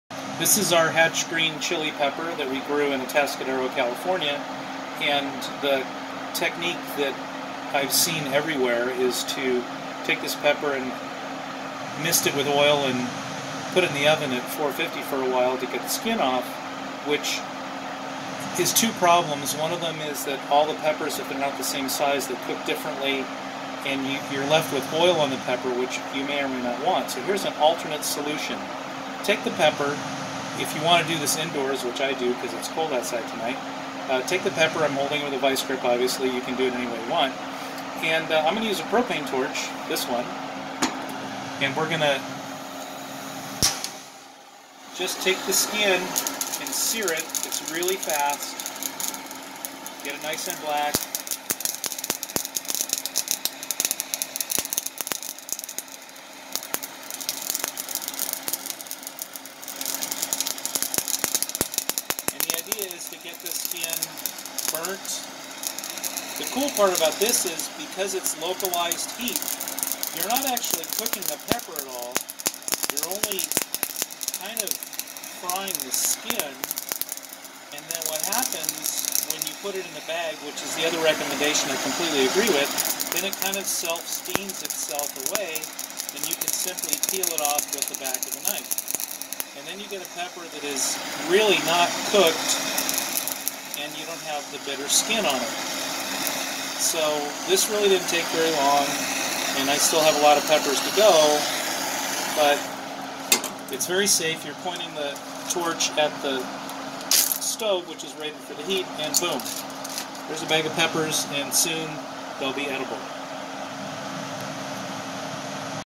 torching peppers
Removing the skin from "Hatch" Green Chilies - alternate to roasting.  Use a propane torch to burn the skin, then place in plastic bag to steam off the bits.  Benefits to using this technique over roasting is that 1) The pepper does not really roast so you get more options for cooking and 2) if your peppers are different in size, you don't end up overcooking the smaller ones to get the larger ones skinned.